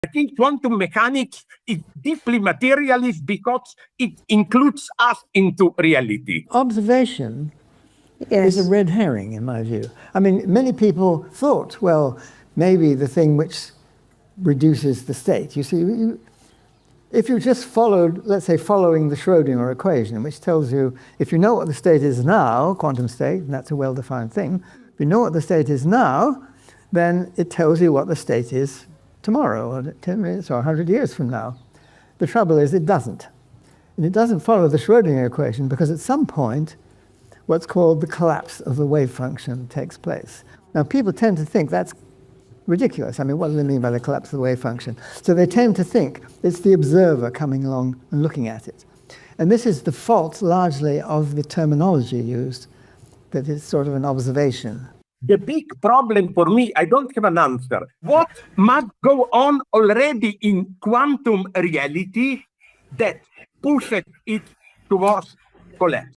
Decoding Quantum Mechanics: The Mystery of Wave Function Collapse
Explore the intricate world of quantum mechanics as a passionate speaker delves into the enigma of wave function collapse. Challenge existing beliefs and engage in a lively discussion about reality and observation in this captivating video. Join us on this enlightening journey!